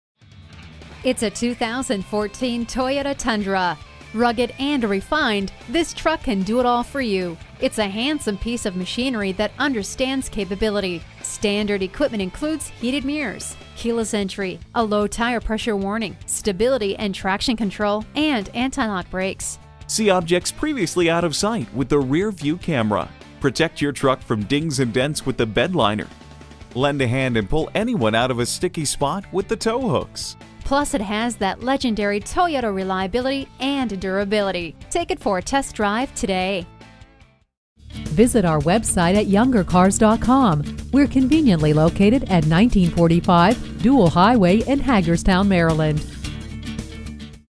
2014 Toyota Tundra Chambersburg-PA Hagerstown, MD T1854800 - SOLD
Year: 2014
Make: Toyota
Model: Tundra
Trim: Double Cab LB 5.7L V8 6-Spd AT SR5
Engine: 8 Cyl. 5.7 L
Transmission: Automatic
Color: Super White
Stock #: T1854800

Address:
1945 Dual Highway